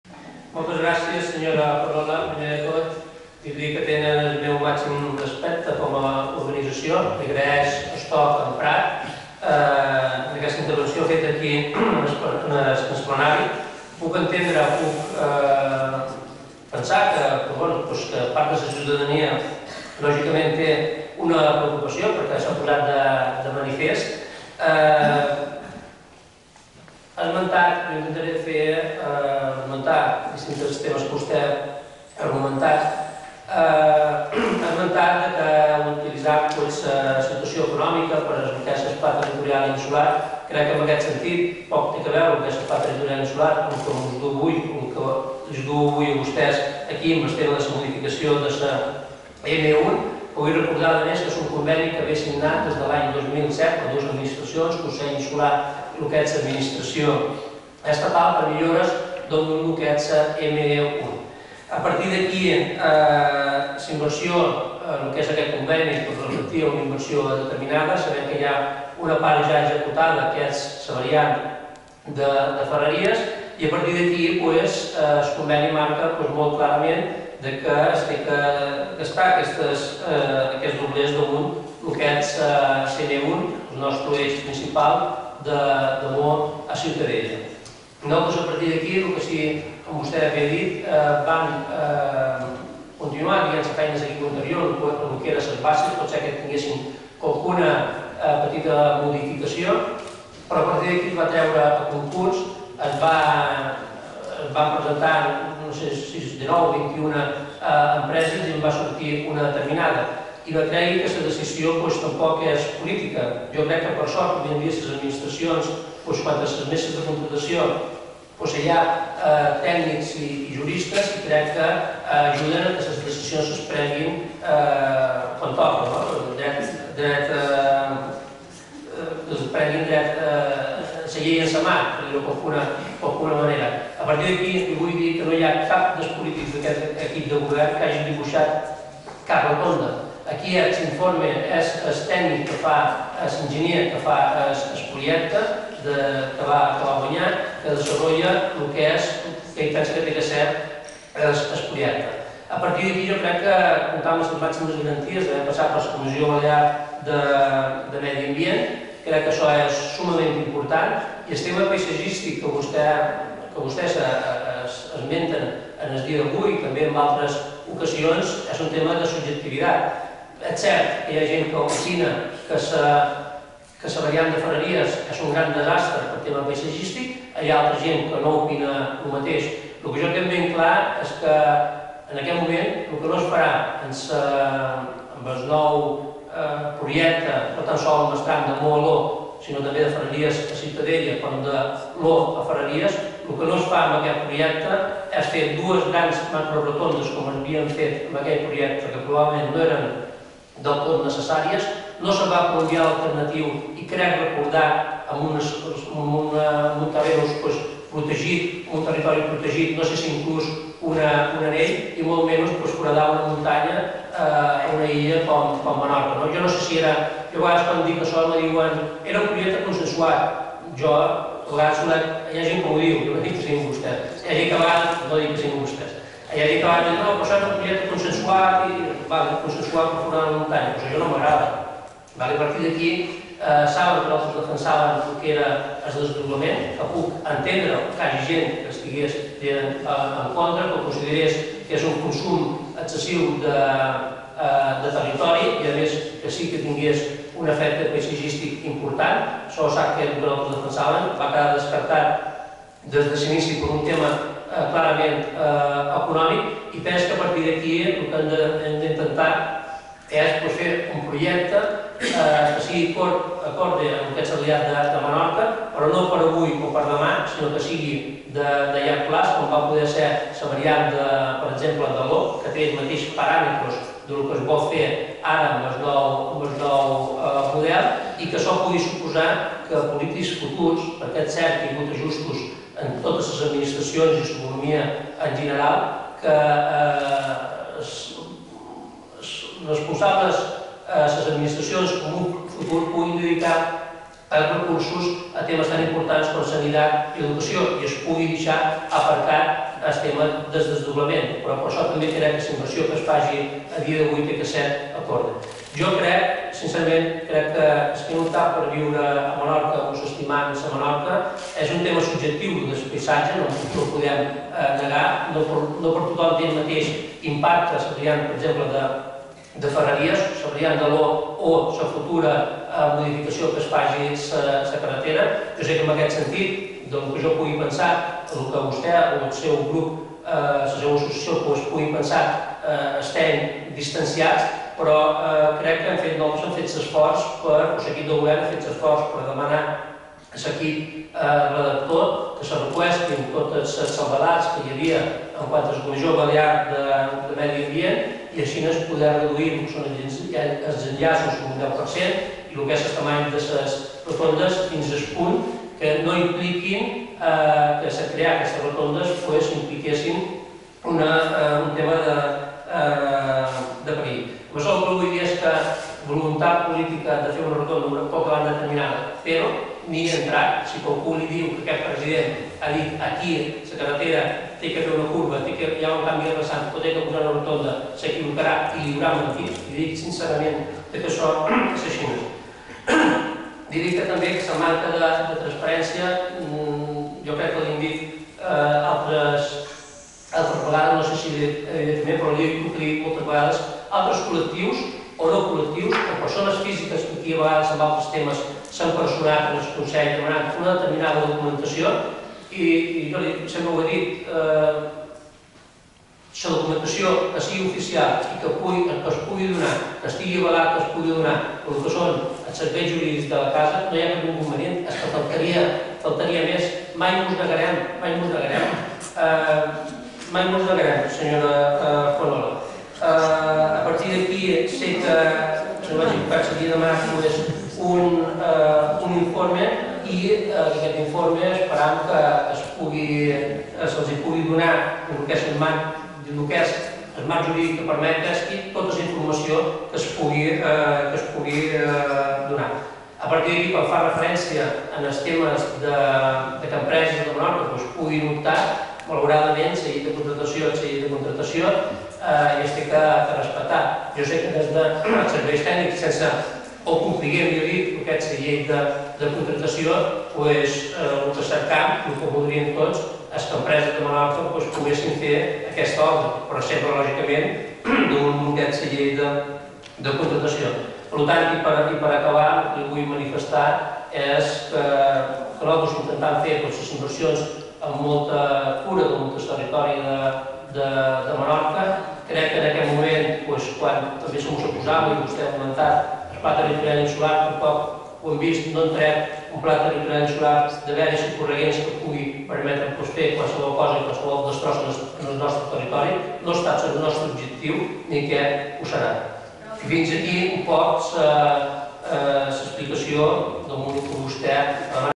Resposta del president del Consell al GOB en relació a la carretera general de Menorca
Més info a savemenorca.org. Aquest vídeo és una resposta a Conxa Juanola, presidenta del GOB: veure